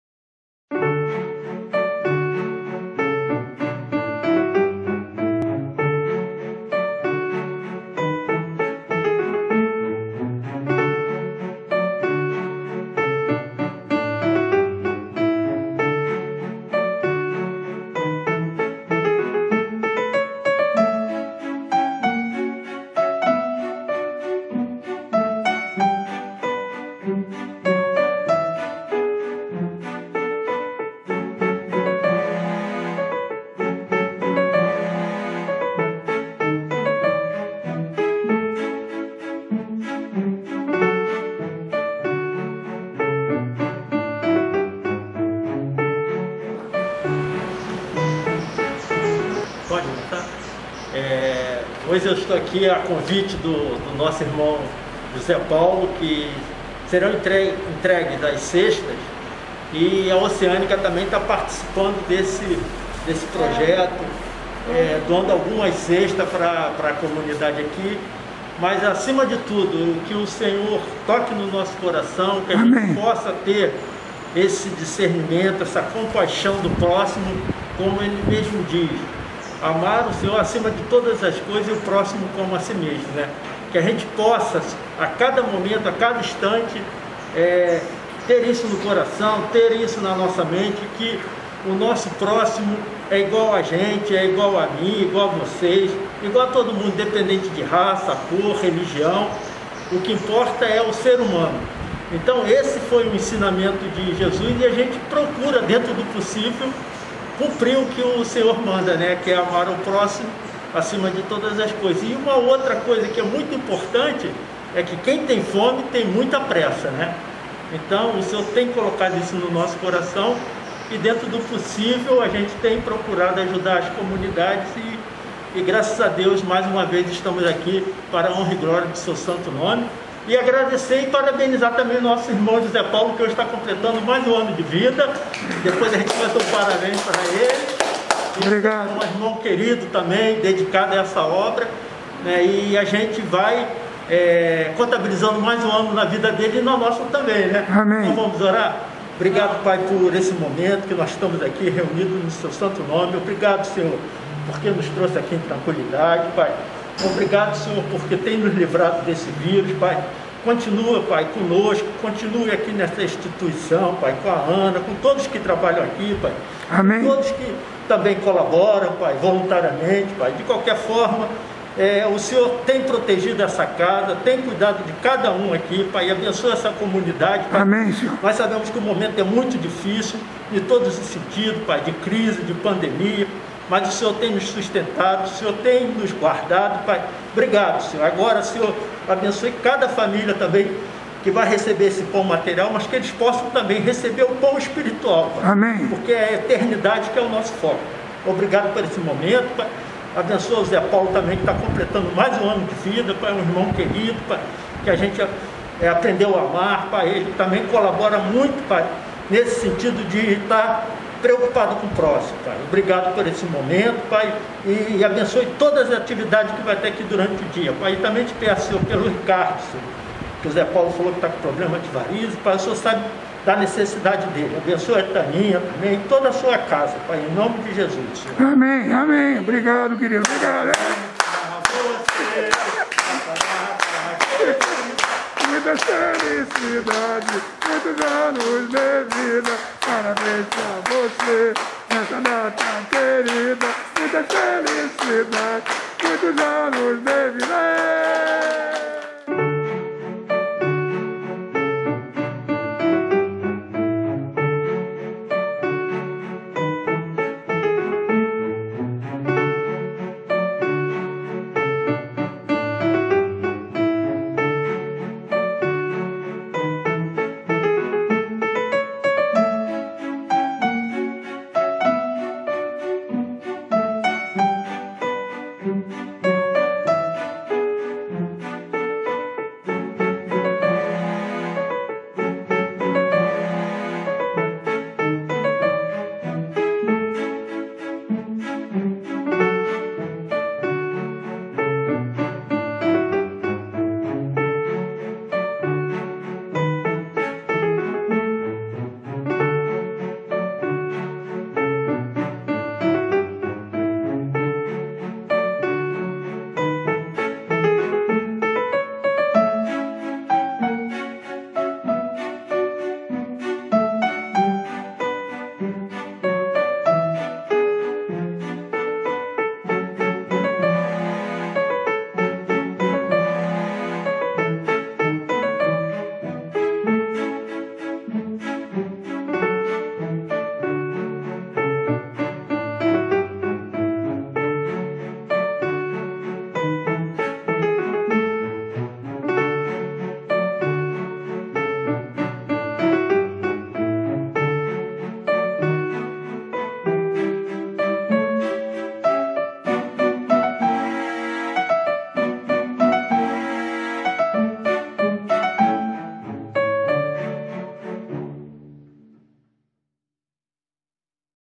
Cestas Básicas FENASE Participando a Ação Social da Igreja Oceânica 08 12 de 2020.
Clips filmados e fotografias pelo fotógrafo Paulo Resende. Tio Paulão.
Câmera Fotográfica Digital CANON EOS Rebel.
Som original. Som local.